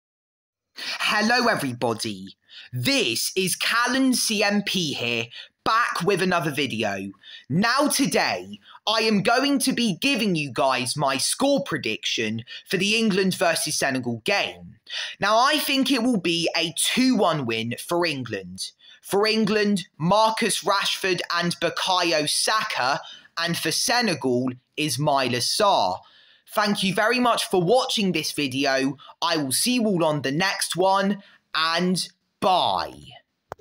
My score prediction for the England vs Senegal game
This seems tight but according to the stats, England have the upper hand. COYTL!